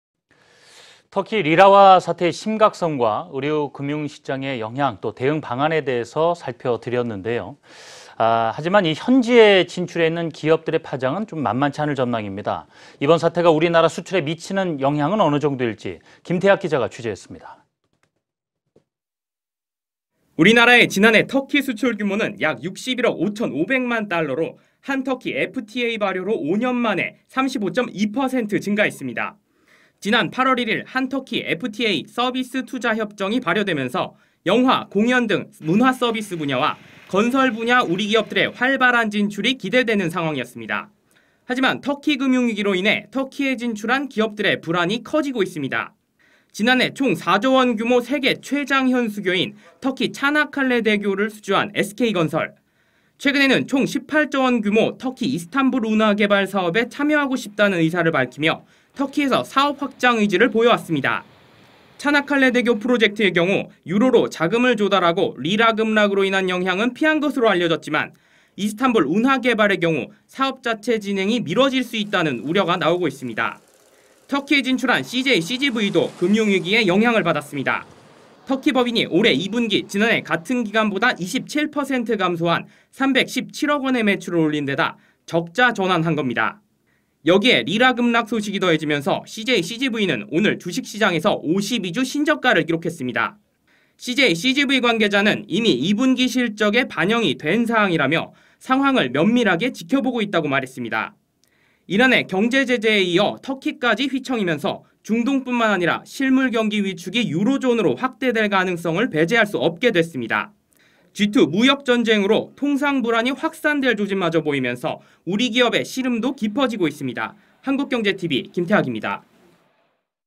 터키발 쇼크에 한국 기업도 '불똥'…건설·영화 사업 차질 / 한국경제TV
[터키발 쇼크에 한국 기업도 '불똥'…건설·영화 사업 차질]

터키 리라화 사태의 심각성과 우리 금융계의 대응 방안에 대해서 살펴보셨는데요.
실물경기에 미칠 파장도 만만치 않을 전망입니다.
이번 사태가 수출에 미치는 영향은 어느 정도일지 김태학 기자가 취재했습니다.